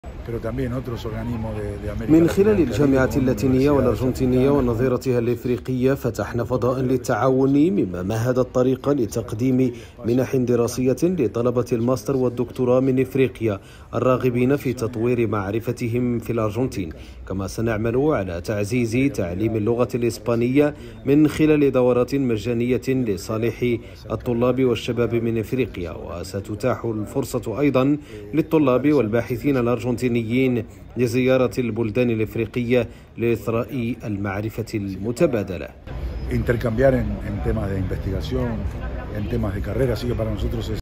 بوينوس آيريس.. التعاون الجامعي في صلب التقارب بين أمريكا اللاتينية وإفريقيا
أبدت جامعات أمريكا اللاتينية وأفريقيا عزمها على بناء مساحات للتبادل والتقارب لتعزيز التعاون جنوب جنوب لصالح البحث العلمي والتنمية.

لمتابعتنا
لتحميل التطبيق
Android